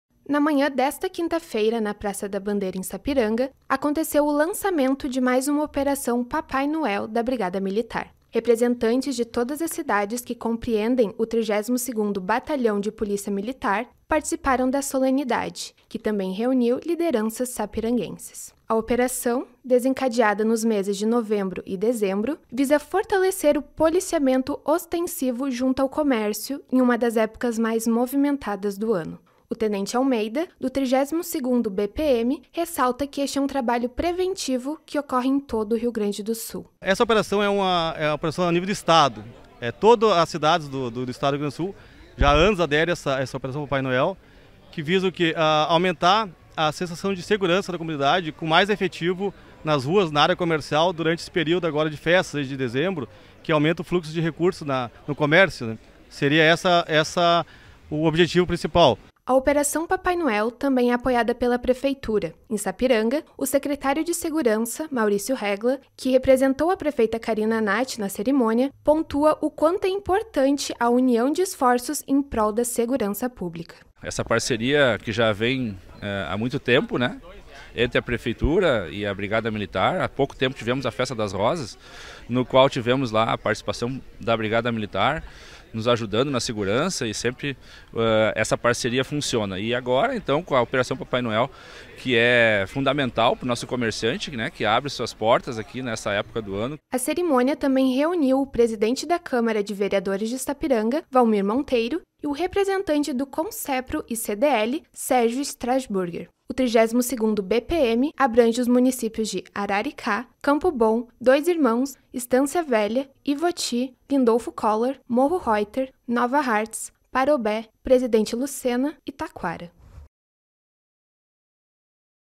POR UM NATAL MAIS SEGURO - Brigada Militar lança mais uma Operação Papai Noel
Matéria e imagens: Melissa Costa - JR

Realização: Grupo Repercussão